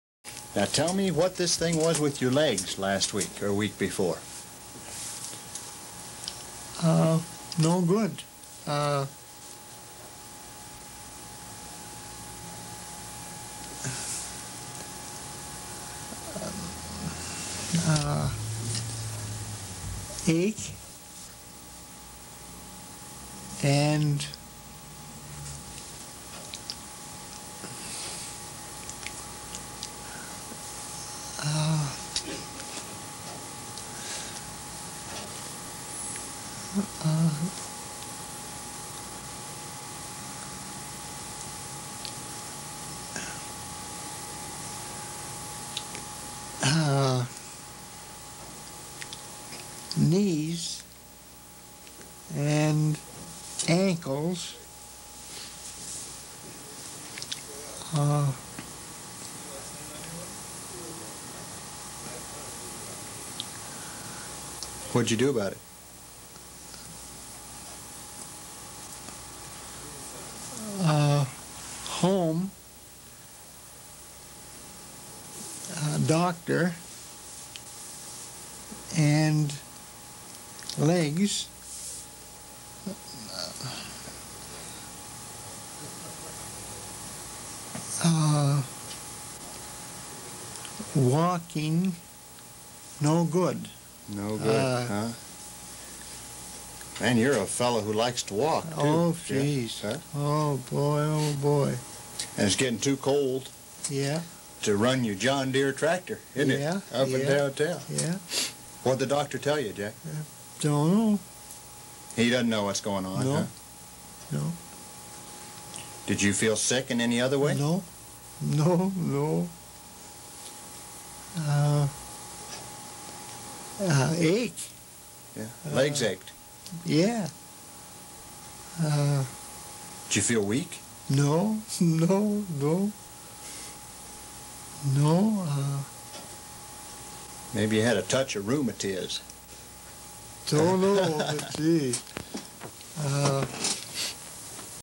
Broca's Aphasia
This is a video of a patient with Broca's aphasia from the Wisconsin Physio Dept website.